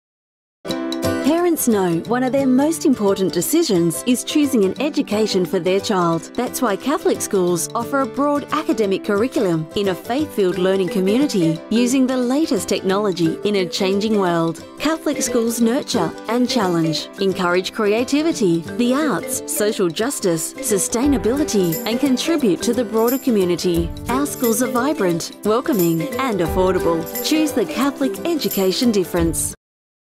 Catholic Education Office Sandhurst TV Commercial
Catholic Education TV Commercial by Shon Productions promoting Catholic Schools in 25% of Victoria.